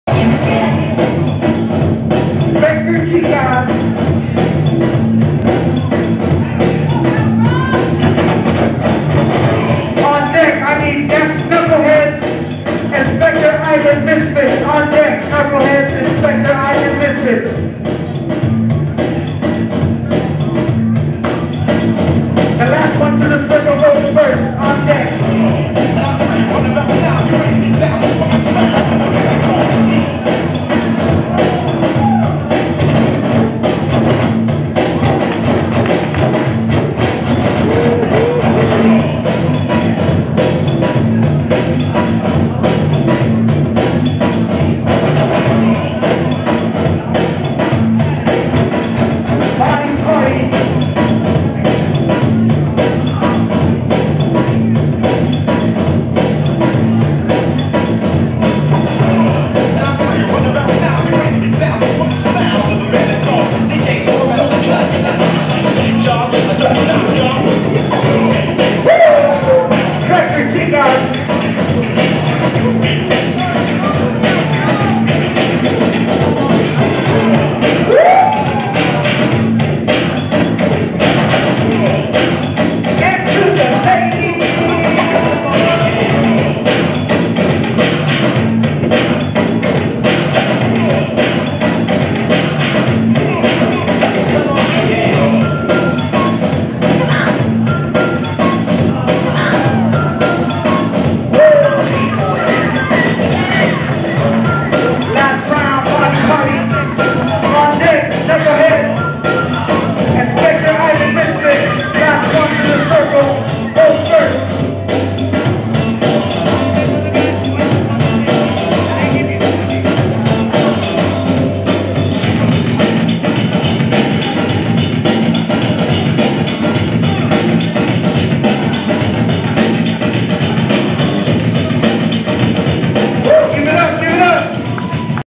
TFK: (Prelims) #2 Specter Chicas vs. Body Party
Prelim 2v2 B-Boy Battles at The Freshest Kingz in San Diego 7/25/14.